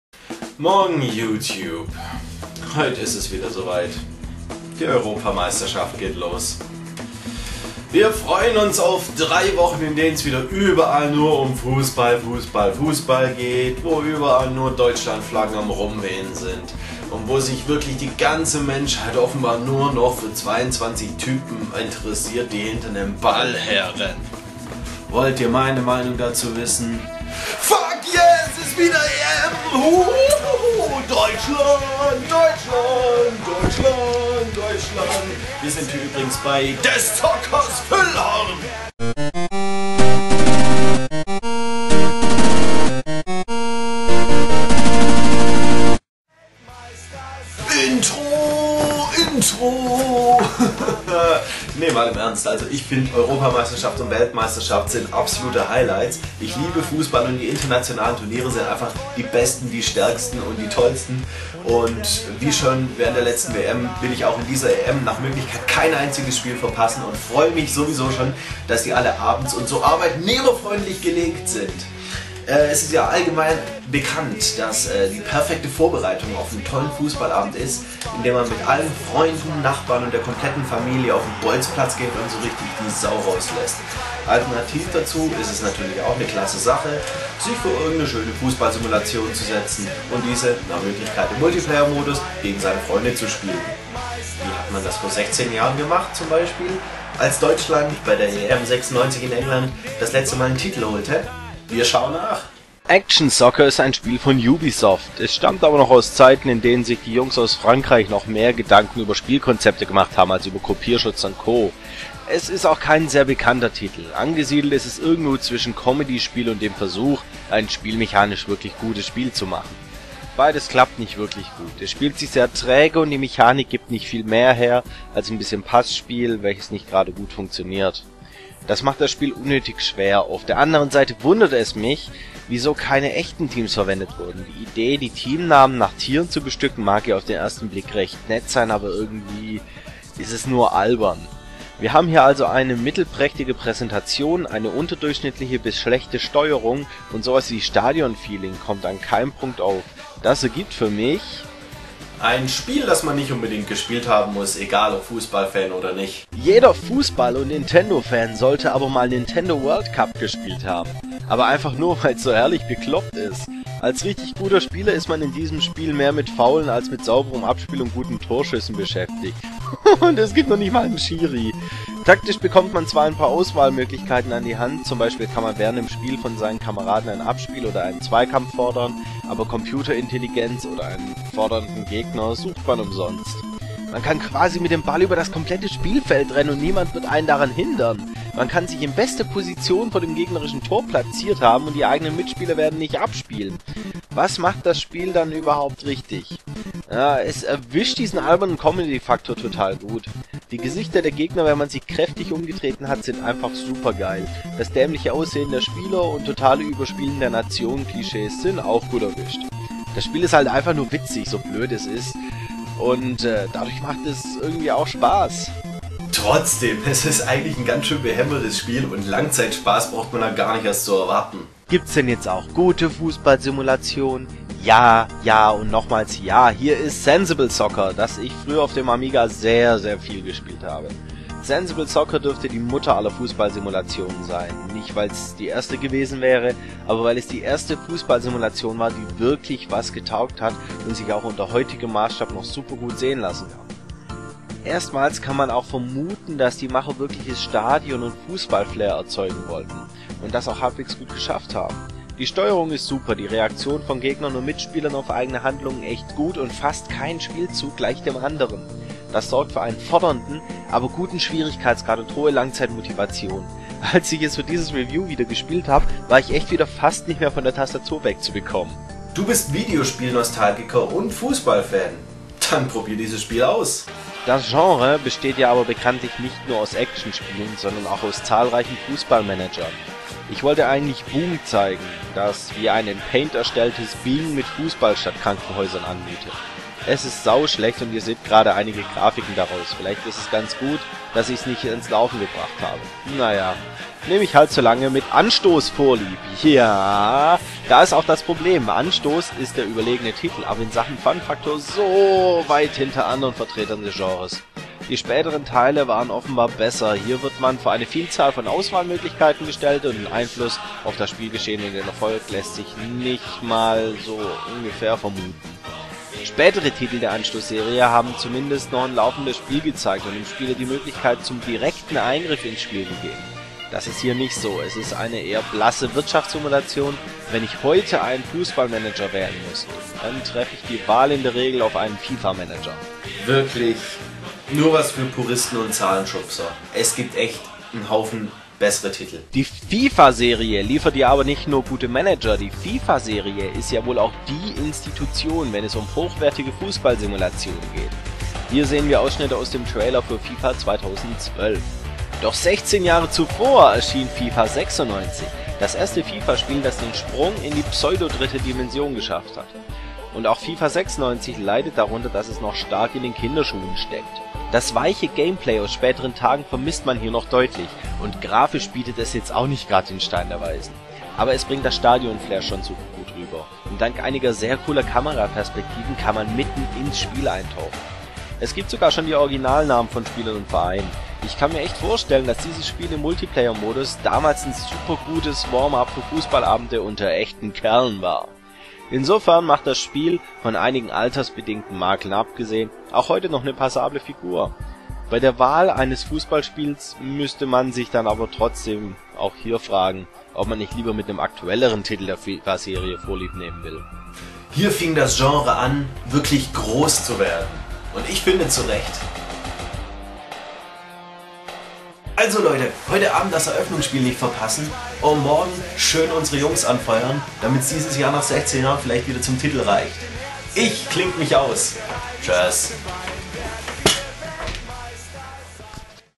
Des Zockers Füllhorn - Retro Fußball-Simulationen
Pünktlich zum EM-Start ein kleines Special über längst vergangene Zeiten, in denen fotorealistische Grafik noch in weiter Ferne lag und das Genre vielmehr auf der Suche nach einem akzeptablen Gameplay war